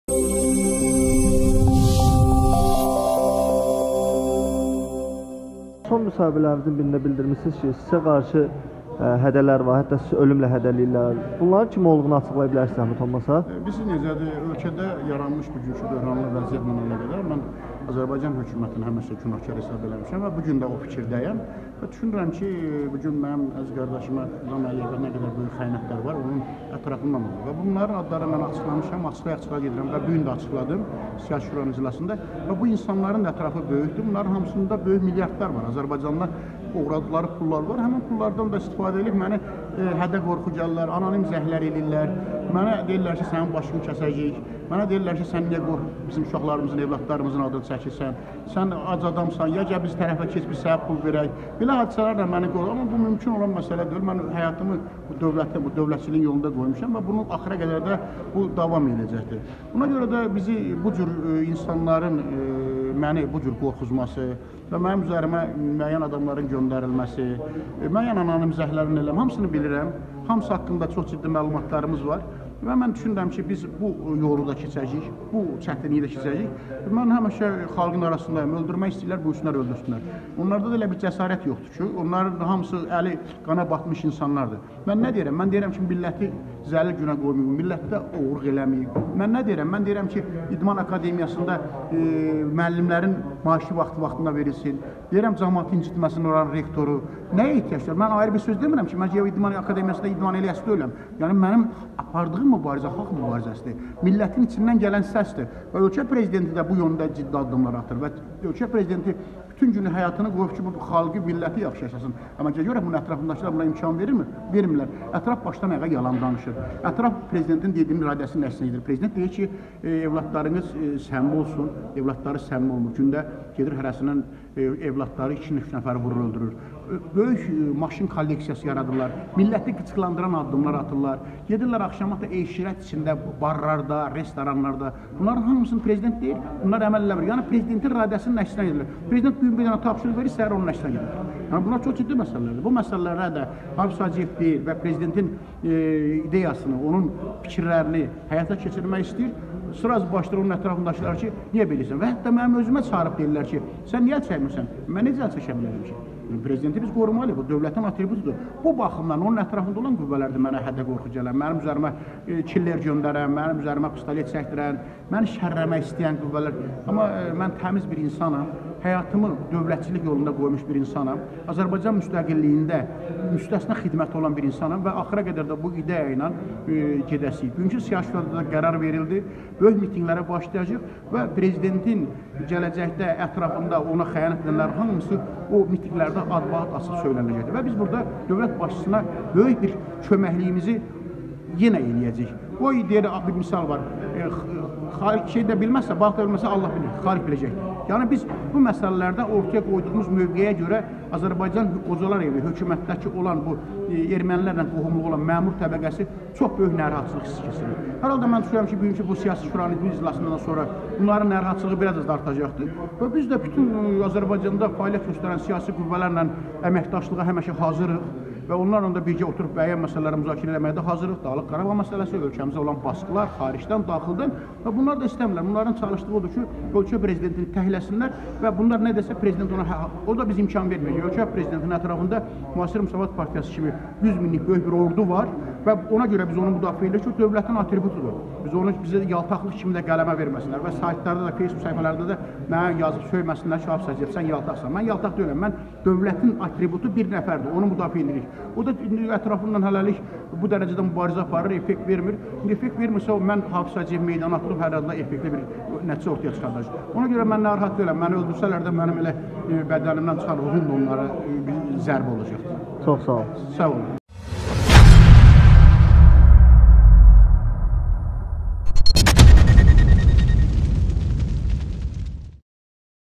"Bəzi məmurlar məni öldürmək üçün killer tutublar"-Hafiz Hacıyev
Yaxın günlərdə bir mətbuat orqanına müsahibə verən Müasir Müsavat partiyasının sədri Hafız Hacıyev ona qarşı təhdidlərin və hədə-qorxuların olduğunu açıqlamışdı.Hətta, Hafız Hacıyev bildirib ki, onu ölümlə də hədələyiblər."Hədələrə baxmayaraq öz fəaliyyətimi davam etdirəcəm və mən öldürsələr də ruhum onlara bəsdir" deyə Hafiz Hacıyev bildirib.